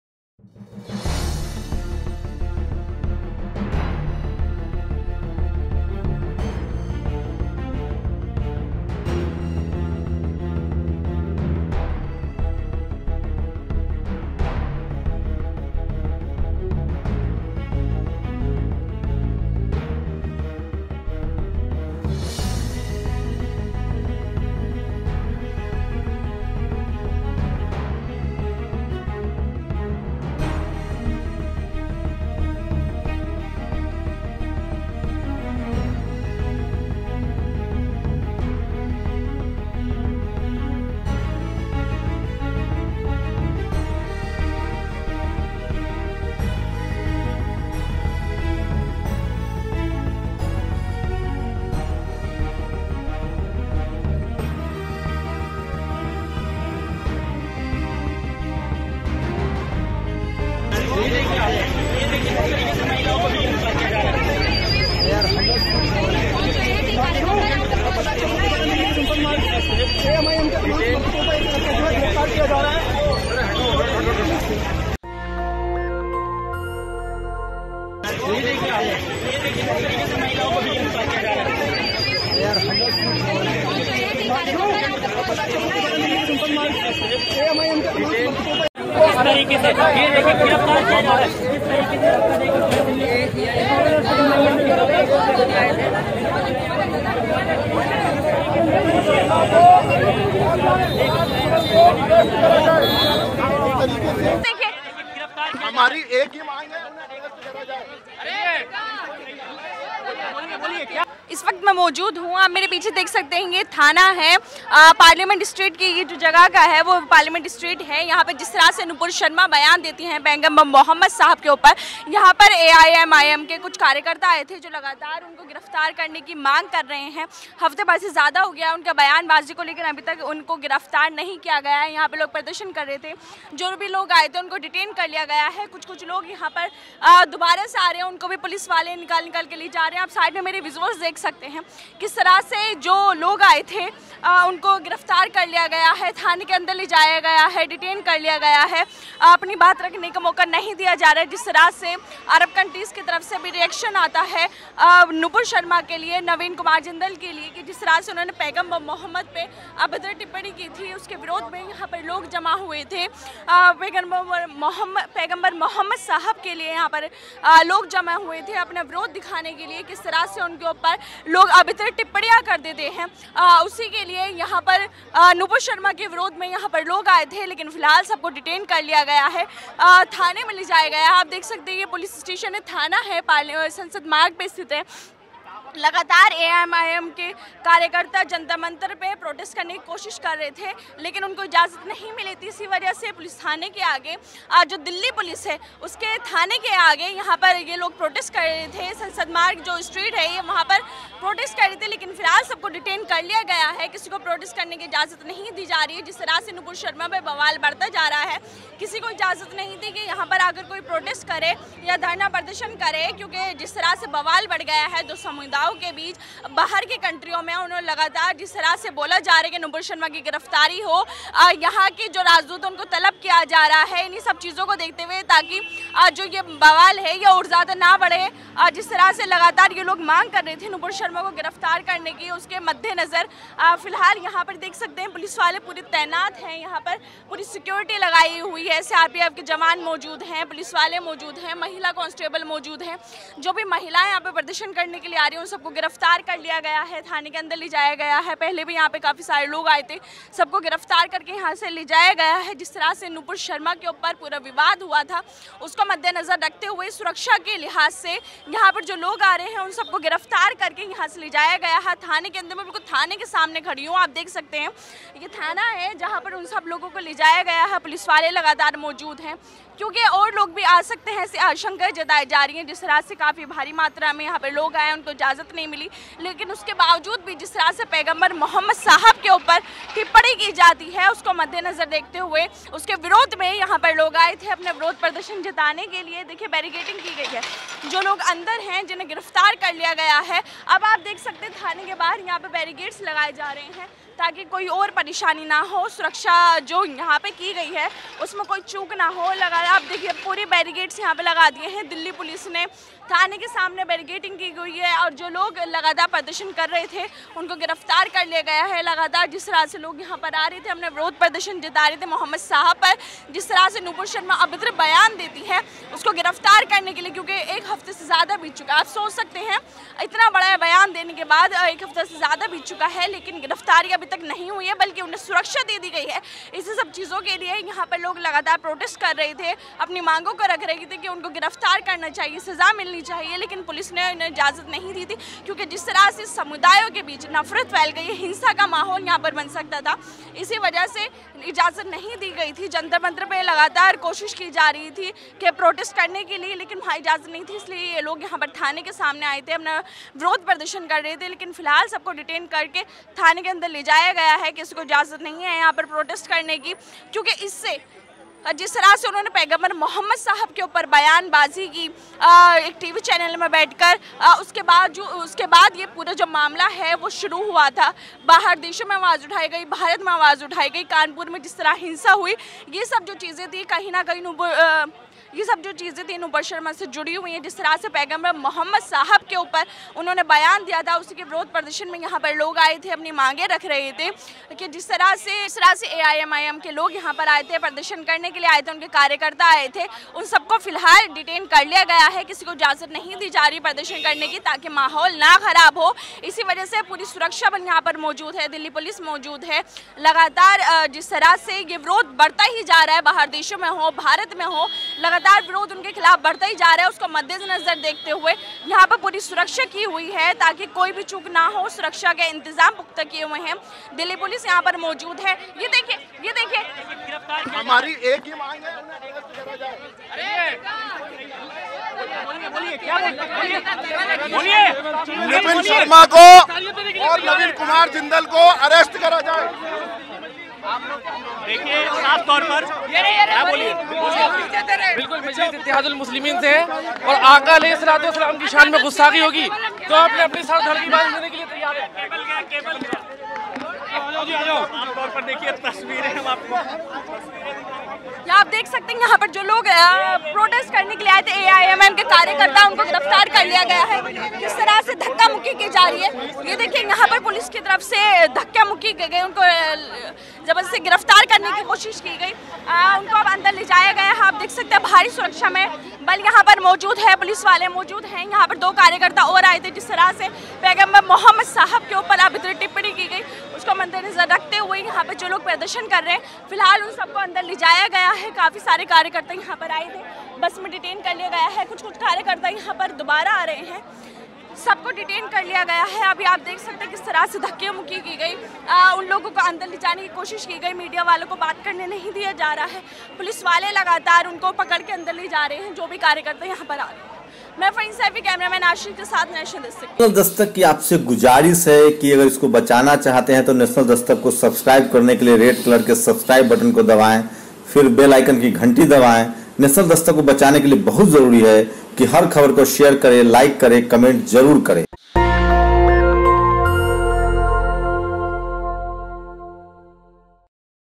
नूपुर शर्मा के खिलाफ ओवैसी की बड़ी जंग/GROUND REPORT ON NUPUR SHARMA WITH FARHEEN
Reporter-FARHEEN SAIFI
Editor-MANUJ
Camera person-ASHISH